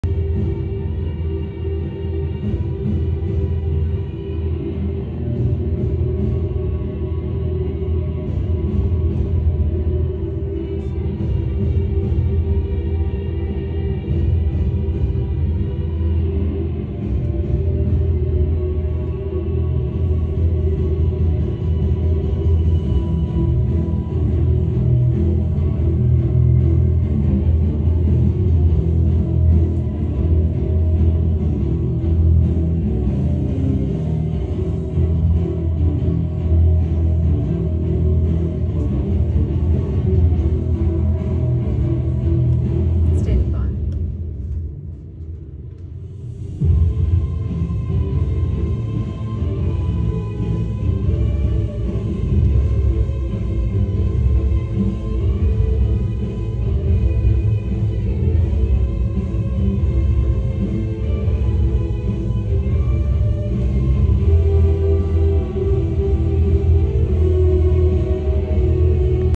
Luxe Orchestra with Electric Cello Solo
Footage of our Luxe Orchestra Feature performance with Solo Electric Cello. Video taken from backstage. November 2023